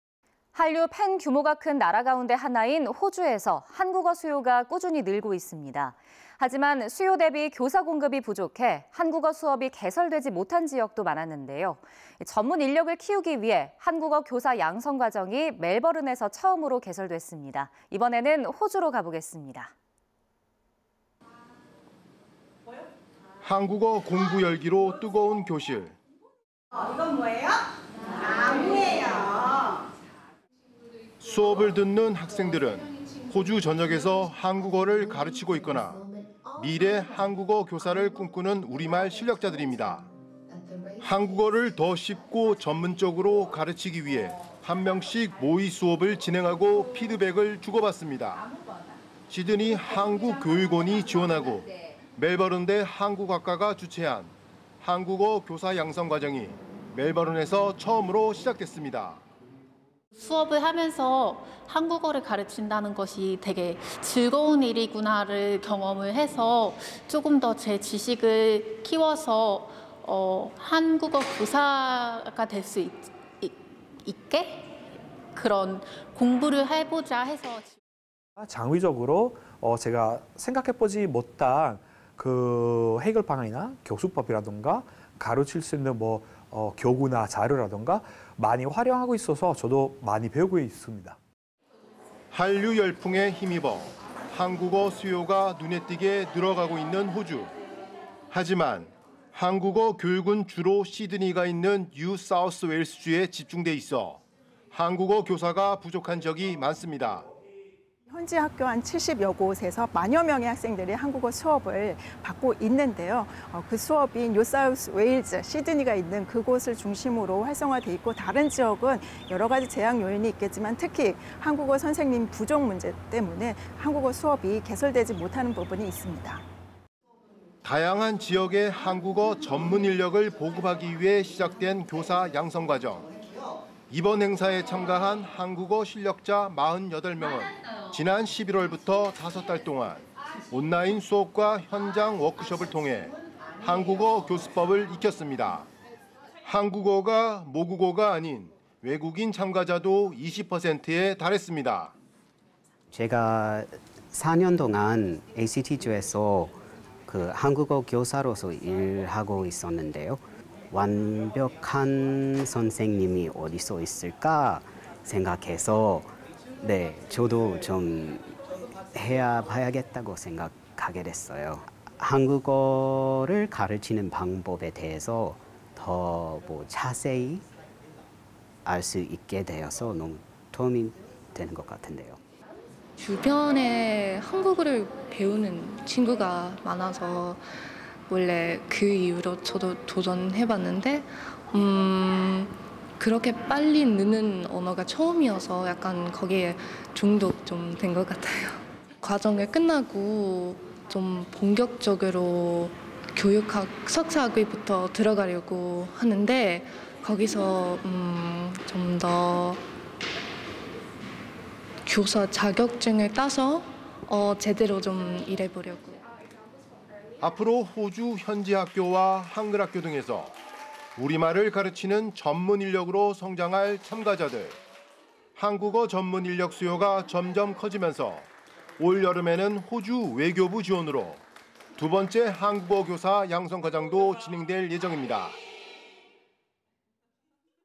"한국어 교육 책임져요"…호주 첫 한국어 교사 양성과정 개설 [글로벌 NOW] / YTN korean
한류 팬 규모가 큰 나라 가운데 하나인 호주에서 한국어 수요가 꾸준히 늘고 있습니다.
하지만, 수요 대비 교사 공급이 부족해 한국어 수업이 개설되지 못한 지역도 많았는데요.
전문 인력을 키우기 위해 한국어 교사 양성 과정이 멜버른에서 처음으로 개설됐습니다.

이번에는 호주로 가보겠습니다.

[기자]
한국어 공부 열기로 뜨거운 교실.

"이건 뭐예요? (나무예요.)"

수업을 듣는 학생들은 호주 전역에서 한국어를 가르치고 있거나 미래 한국어 교사를 꿈꾸는 우리말 실력자들입니다.

한국어를 더 쉽고 전문적으로 가르치기 위해 한 명씩 모의 수업을 진행하고 피드백을 주고받습니다.

시드니 한국교육원이 지원하고 멜버른대 한국학과가 주최한 한국어 교사 양성 과정이 멜버른에서 처음으로 시작됐습니다.

[이수민 / 호주 애들레이드·프로그램 참가자 : 수업하면서 한국어를 가르친다는 것이 즐거운 일이라는 걸 경험해서 조금 더 제 지식을 키워서 한국어 교사가 될 수 있게 공부를 해보자 해서….]

[니콜라 프라스키니 / 멜버른대학교 한국학과 부교수·프로그램 담당자 : (참가자들이) 창의적으로 제가 생각해보지 못한 해결 방안이나 교수법이라든가 가르칠 수 있는 교구나 자료를 많이 활용하고 있어서 저도 많이 배우고 있습니다.]

한류 열풍에 힘입어 한국어 수요가 눈에 띄게 늘어가고 있는 호주.

하지만, 한국어 교육은 주로 시드니가 있는 뉴사우스웨일스주에 집중돼있어, 한국어 교사가 부족한 지역이 많았습니다.

[권지영/ 시드니 한국교육원장 : 현지 학교 70여 곳에서 1만여 명의 학생들이 한국어 수업을 받고 있는데요. 그 수업이 뉴사우스웨일스주, 시드니가 있는 그곳을 중심으로 활성화돼 있고 다른 지역은 여러 가지 제약 요인이 있겠지만, 특히 한국어 선생님 부족 문제 때문에 한국어 수업이 개설되지 못하는 부분이 있습니다.]

다양한 지역에 한국어 전문 인력을 보급하기 위해 시작된 교사 양성 과정.

이번 행사에 참가한 한국어 실력자 48명은 지난 11월부터 다섯 달 동안 온라인 수업과 현장 워크숍을 통해 한국어 교수법을 익혔습니다.

한국어가 모국어가 아닌 외국인 참가자도 20%에 달했습니다.

[심향후/ 호주 캔버라·프로그램 참가자 : 제가 4년 동안 ACT주(호주 수도 특별구)에서 한국어 교사로서 일하고 있는데요. 완벽한 선생님은 어떻게 될 수 있을까 생각해서 저도 해봐야겠다고 생각했어요./ 0246 한국어를 가르치는 방법에 대해서 더 자세히 알 수 있게 되어서 너무 도움이 되는 것 같아요.]

[클로이 로즈 엘리스/ 호주 퍼스·프로그램 참가자 : 주변에 한국어 배우는 친구가 많아서 원래 그 이후로 저도 도전해봤는데 이렇게 빨리 느는 언어가 처음이어서 거기에 중독된 것 같아요. 과정을 끝내고 본격적으로 교육학 석사학위부터 들어가려고 하는데 거기서 교사 자격증을 따서 제대로 (한국어 교사로) 일해보려고….]

앞으로 호주 현지 학교와 한글학교 등에서 우리말을 가르치는 전문 인력으로 성장할 참가자들.

한국어 전문 인력 수요가 점점 커지면서 올여름에는 호주 외교부 지원으로 두 번째 한국어 교사 양성 과정도 진행될 예정입니다.